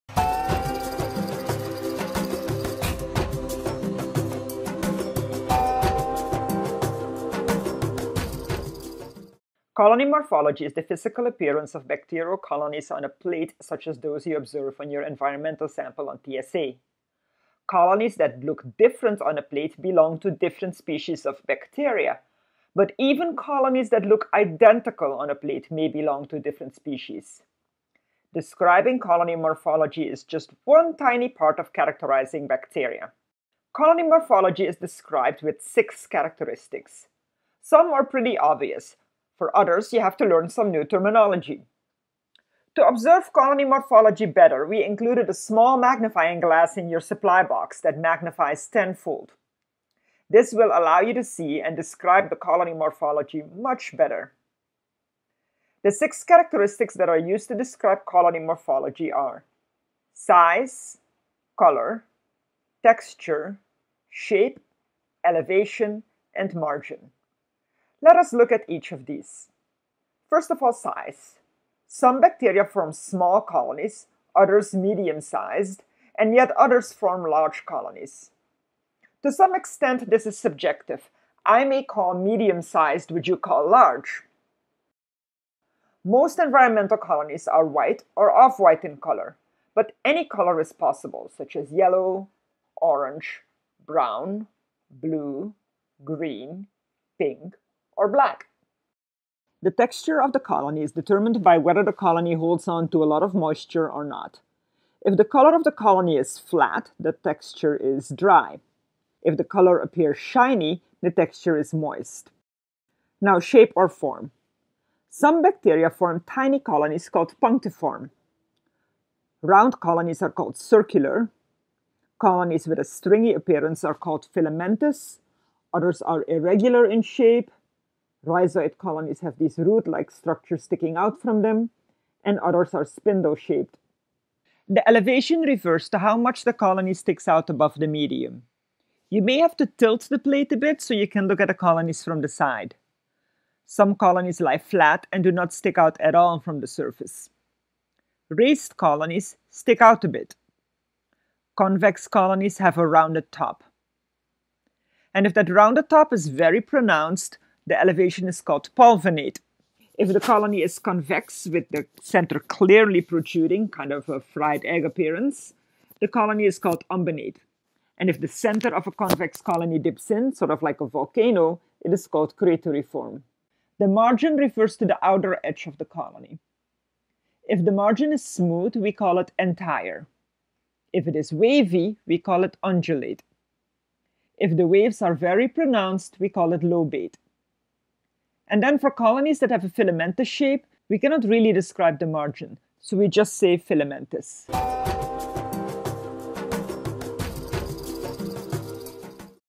Describing colony morphology
This video explains the six characteristics that students in the Microlab-At-Home program use to describe colony morphology.